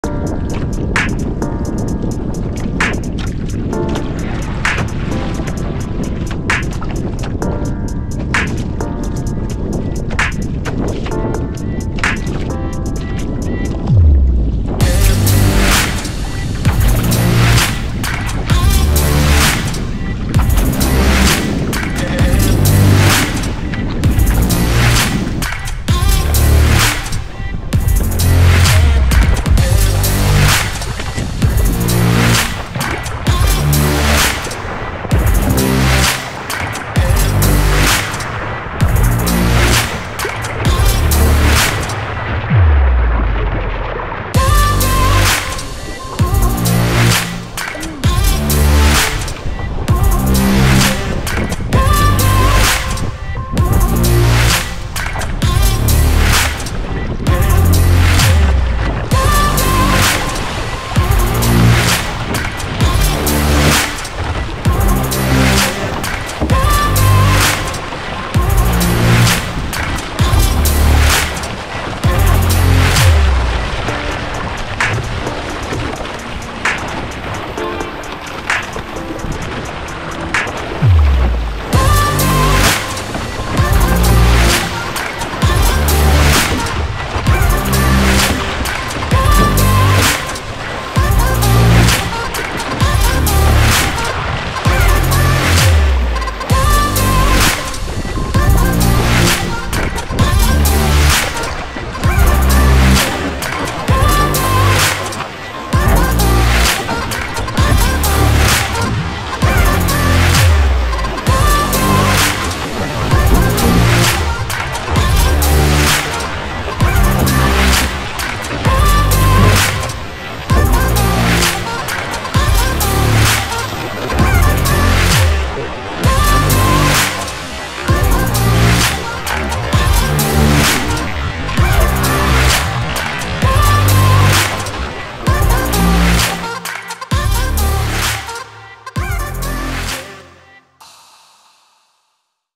K Bay OC1 Surf Session
Banging around outside the channel looking for Waves.  Grabbed a few. It was windy, bumpy but still fun and worth the paddle out in such amazing waters!